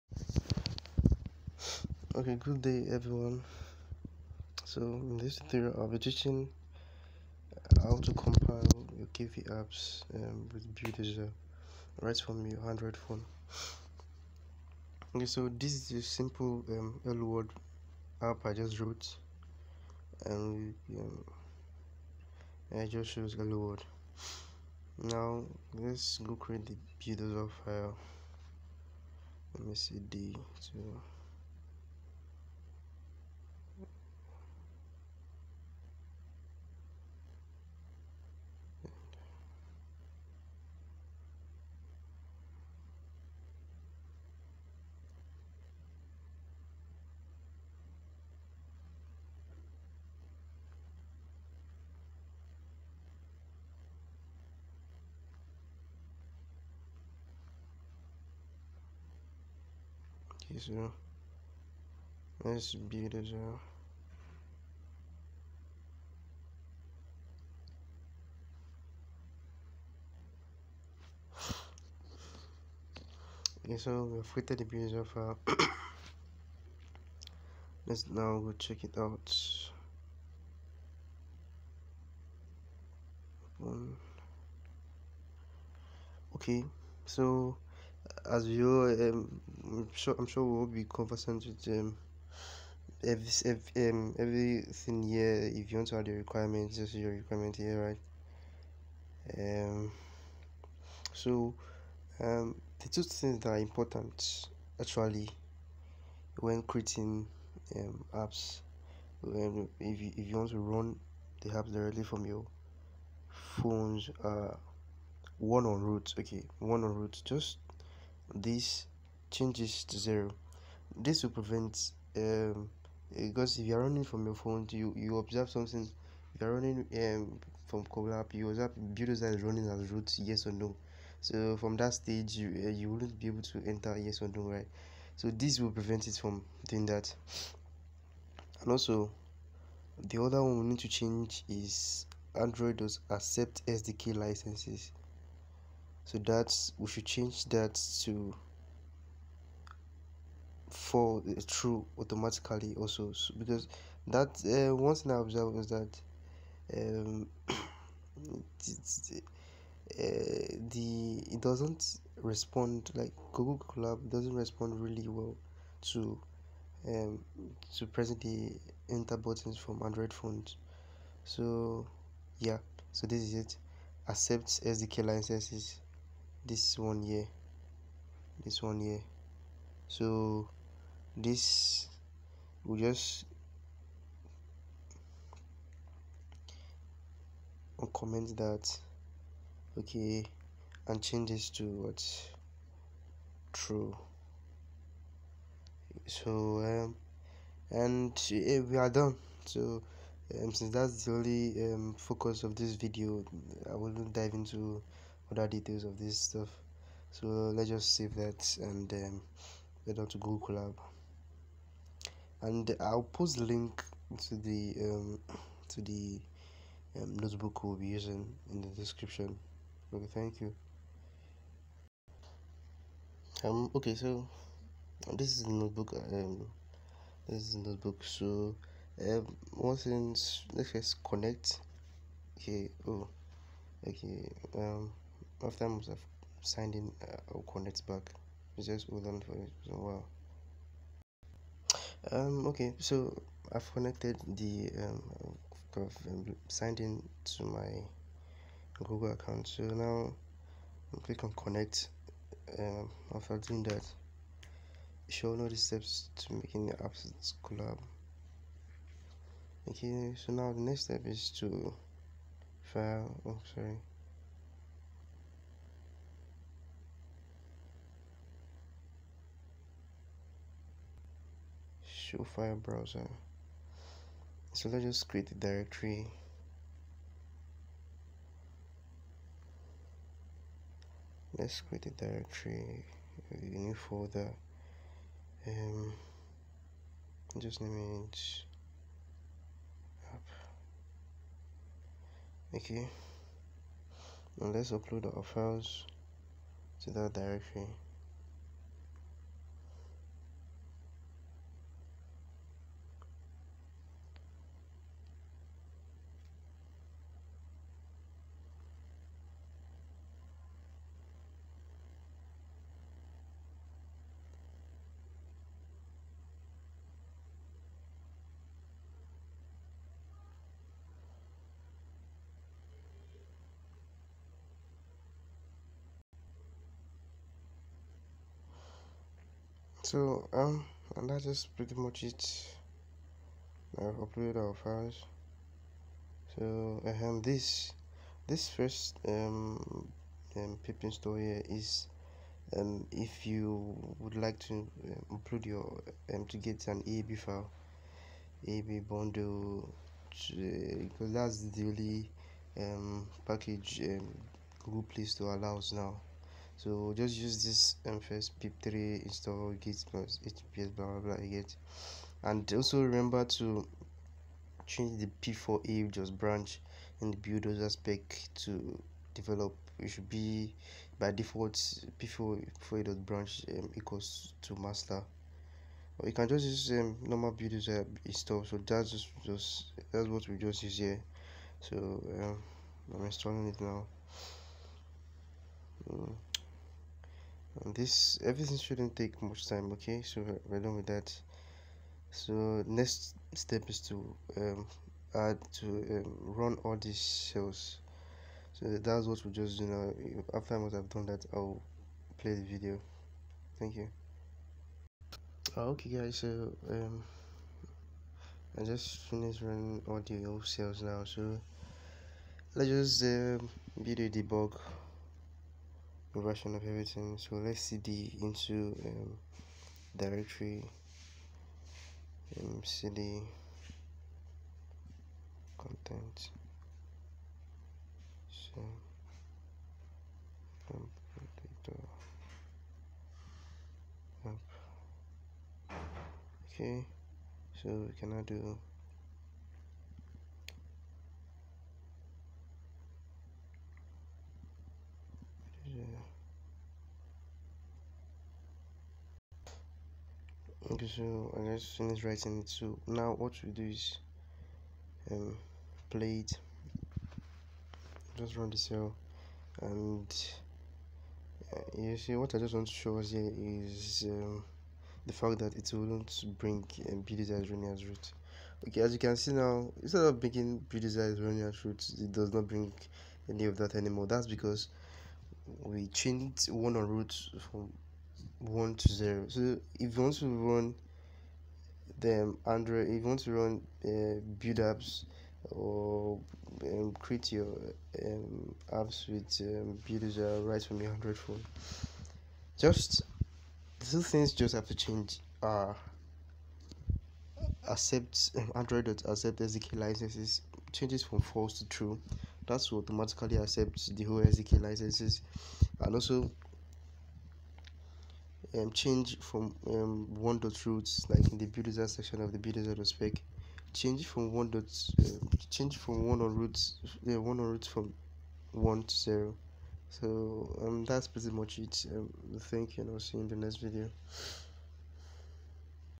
Compile Android Apps Using Google Colab with Your Smart Phone - You can build Apps with your phone 😎
This video would tell how to compile android apps using buildozer in google colab.
Particularly, it tells how to bypass the "Buildozer is running as root" dialogue.
The link to the note book used is
Thanks.